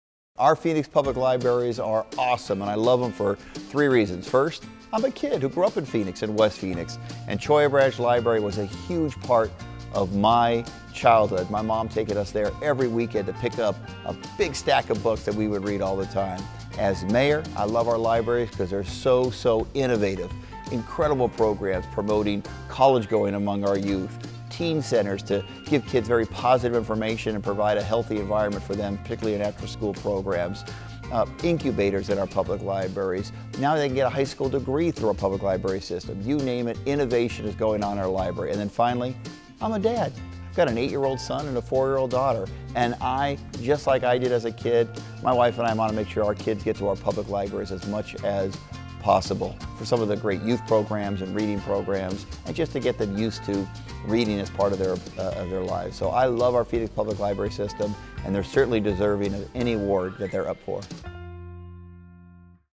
Mayor Stanton Loves Phoenix Public Libraries
Phoenix Mayor Greg Stanton shares all the reasons why he loves the Phoenix Public Library, from his childhood visits to Cholla Library to current day as a parent of two young children.